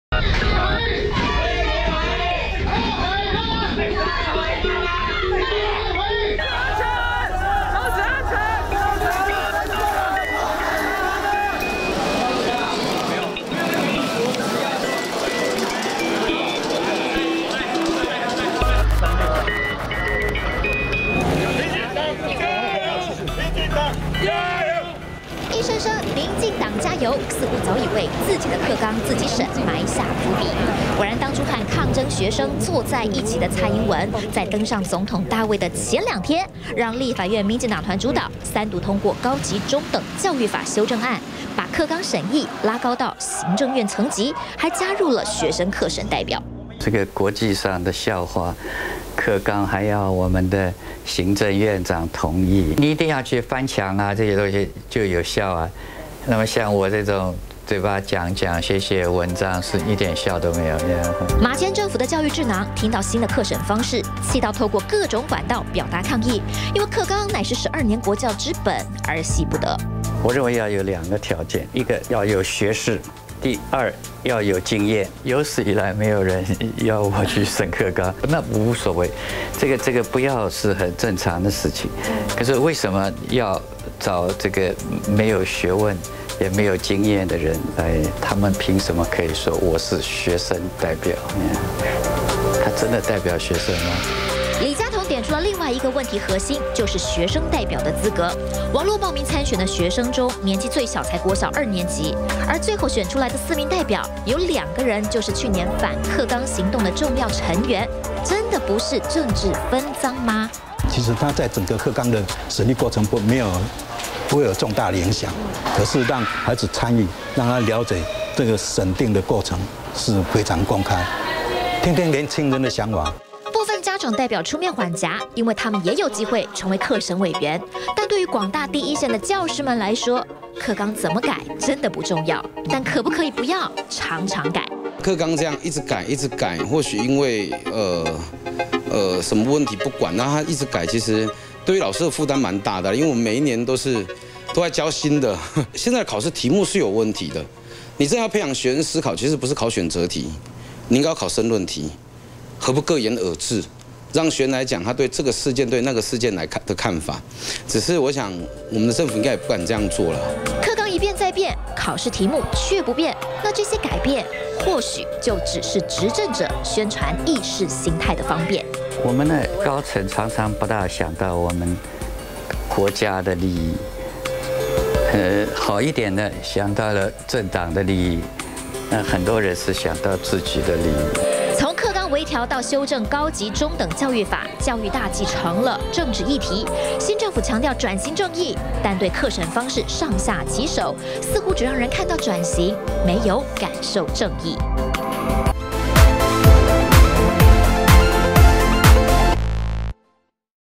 20160828中天新聞 ｢自己的課綱自己審｣成真 李家同批大笑話
反課綱行動，在｢高級中等教育法｣修正案三讀通過後，有了新方向：課綱審定小組確定加入學生代表！而四名學生代表有兩個人就是去年反課綱運動的核心成員，讓教育大計又成了政治議題，教授李家同也因此痛批，新政府是在鬧「國際笑話」！